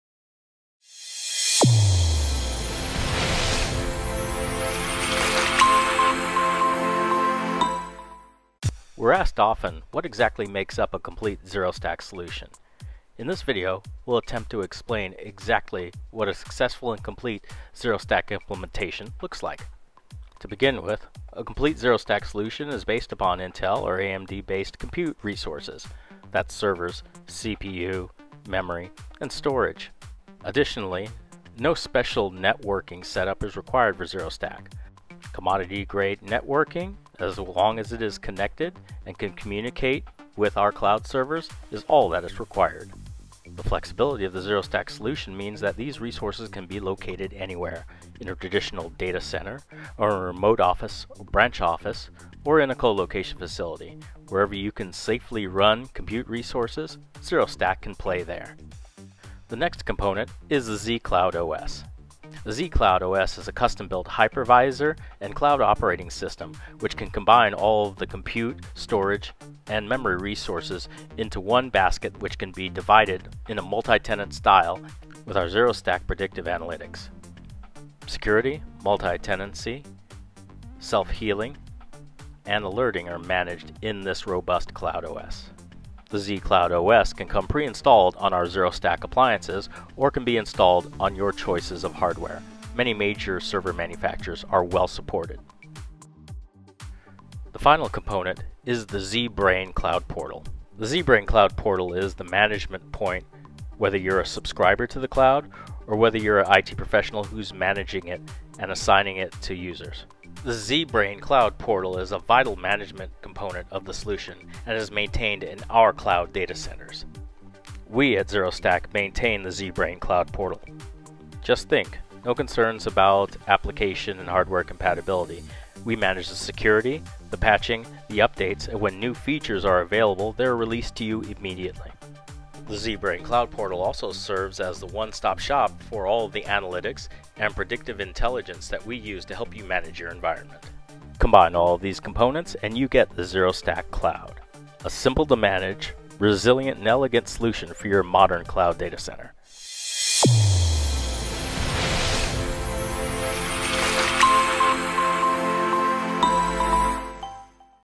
ZeroStack - What is the ZeroStack Cloud?
This video is about ZeroStack - Infrastructure and Concept Explainer